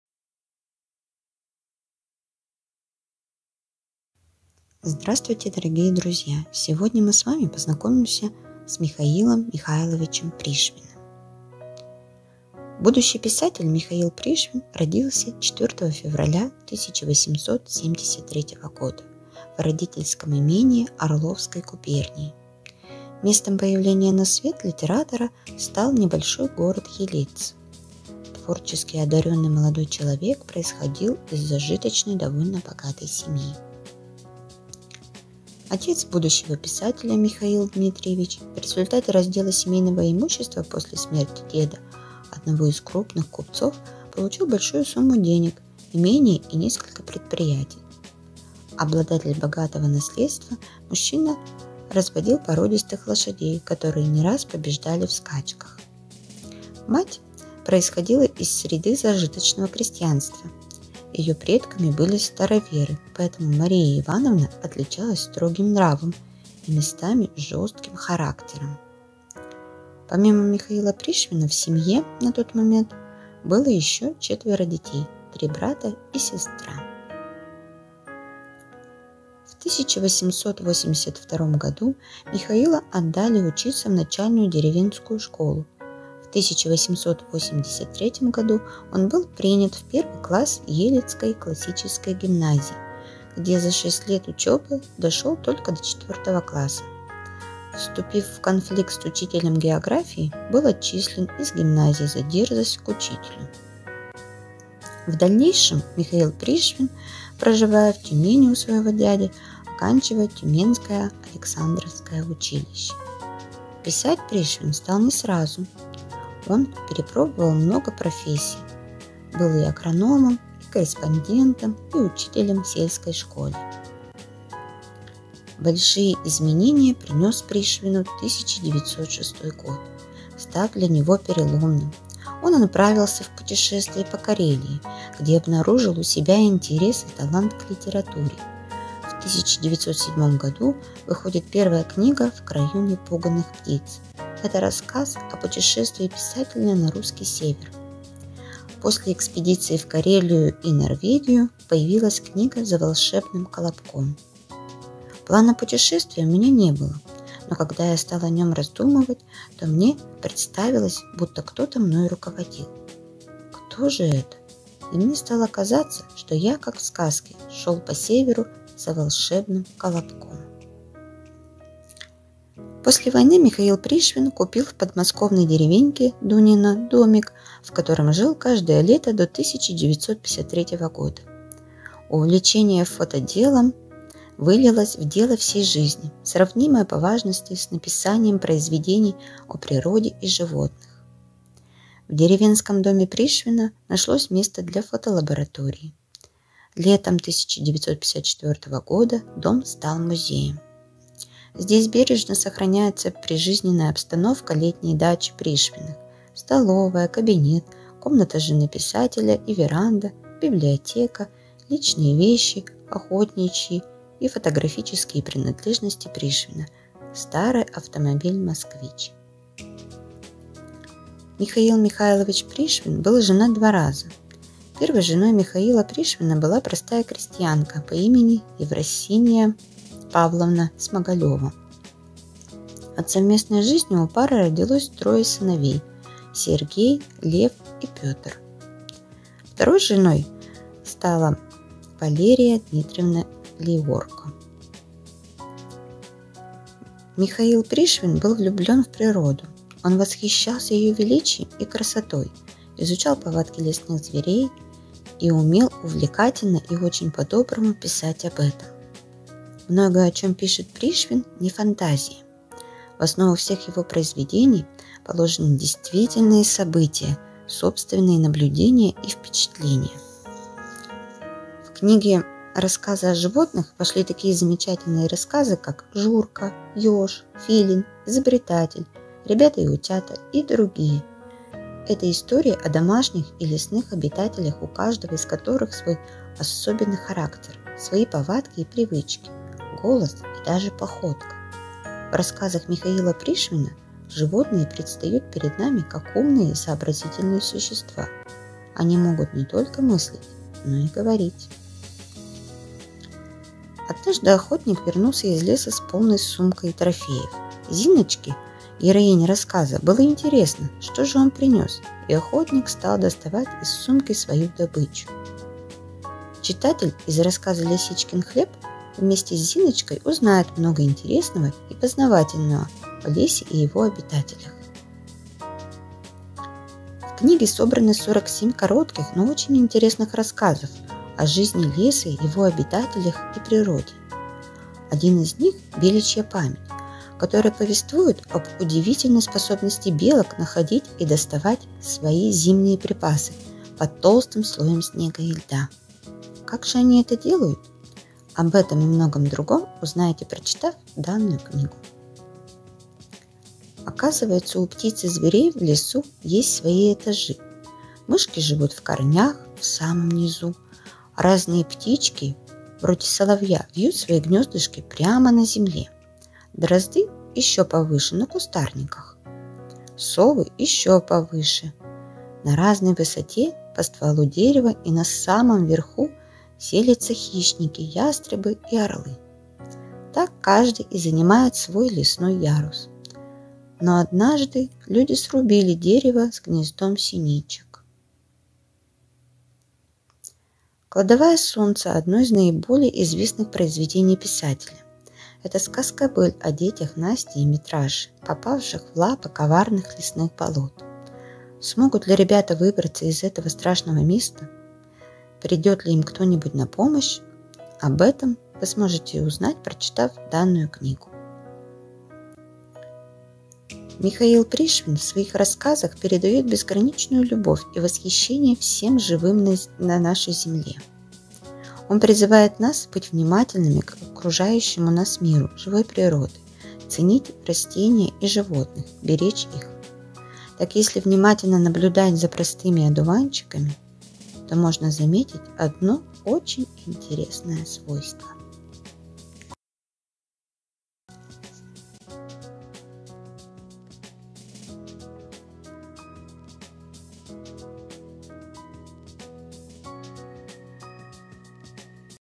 Добро пожаловать в мир Пришвина
Варавинская детская библиотека № 11 представляет для юных читателей видеопрезентацию про детского писателя.